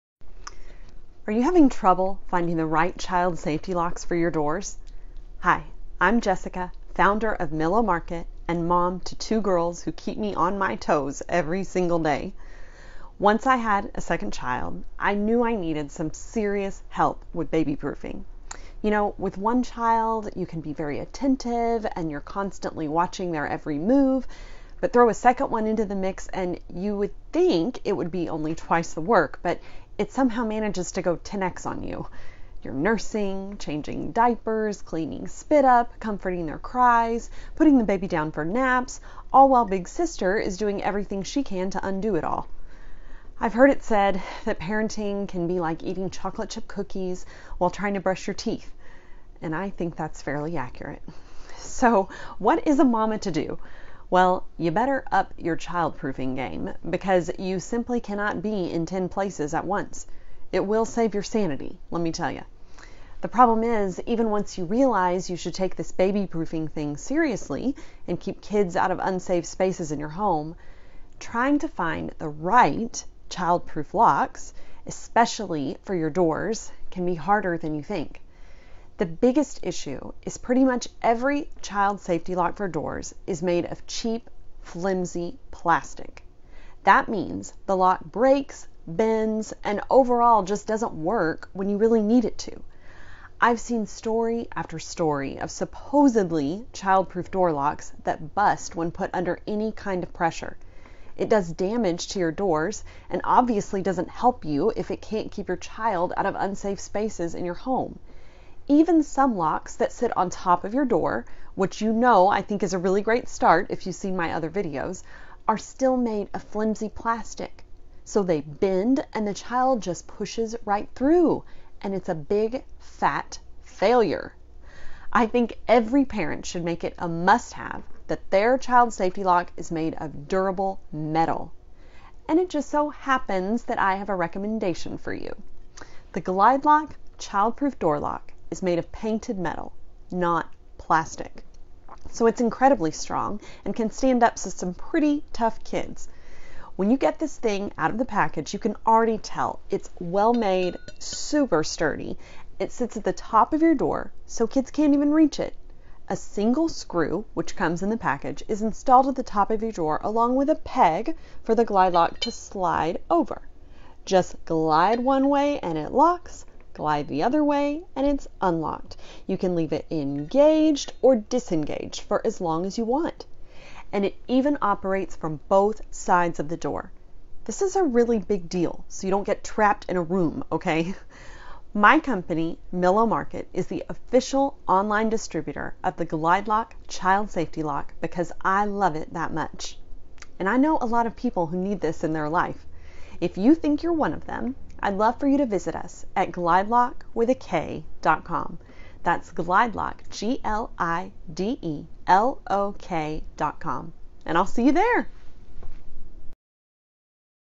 Is Your Child Proof Door Lock Made of THIS?
Are you having trouble finding the right child safety locks for your doors? Hi, I’m Jessica, founder of Millow Market and mom to two girls who keep me on my toes every single day.

Once I had my second child, I knew I needed some serious help with baby proofing. You know, with one child, you can be very attentive and you’re constantly watching their every move. Throw a second one into the mix, and you’d think it would only be twice the work, but it somehow manages to go 10x on you.

You’re nursing, changing diapers, cleaning spit-up, comforting their cries, putting the baby down for naps – all while big sister is doing everything she can to undo it all. I’ve heard it said that parenting can be like eating chocolate chip cookies while trying to brush your teeth … I think that’s fairly accurate.

So what is a mama to do? Well, you better up your childproofing game. Because you simply cannot be in 10 places at once. It will save your sanity, let me tell ya. The problem is, even once you realize you should take this babyproofing thing seriously and keep kids out of unsafe spaces in your home, trying to find the right child proof locks – especially for your doors – can be harder than you think.

The biggest issue is pretty much every child safety lock for doors is made of cheap, flimsy plastic. That means the lock breaks, bends and overall just doesn’t work when you really need it to. I’ve seen story after story of supposedly child proof door locks that bust when put under any kind of pressure. It does damage to your doors, and obviously doesn’t help you if it can’t keep your child out of unsafe spaces in your home.

Even some locks that sit on top of your door – which you know I think is a really great start if you’ve seen my other posts – are still made of flimsy plastic. So they bend and the child just pushes right through. And it’s a big. Fat. Failure. I think every parent should make it a must-have that their child safety lock is made of durable METAL. And it just so happens that I have a recommendation for you.

The GlideLok child proof door lock is made of painted metal – NOT PLASTIC. So it is incredibly strong and can stand up to some pretty tough kids. When you get this thing out of the package, you can already tell it’s well made and super sturdy.

It sits at the top of your door, so kids can’t even reach it. A single screw is installed at the top of your door along with a peg for the GlideLok to slide over. Just glide one way, and it locks. Glide the other way, and it’s unlocked. You can leave it engaged or disengaged for as long as you want.

And it even operates from BOTH sides of the door. This is a really big deal so you don’t get trapped in a room, OK? My company, Millow Market, is the official online distributor of the GlideLok child safety lock because I love it that much! And I know a lot of people who need this in their life. If you think you’re one of them, I would love for you to visit us And I'll see you there!